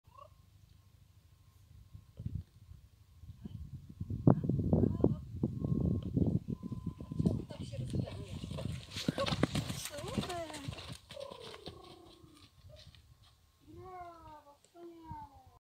Zabawy na cordeo - skok i zatrzymanie przed drążkiem
Brawo Iskierka! A teraz rzucam wyzwanie: kto zrobi to ćwiczenie na Norce?  :) :) :)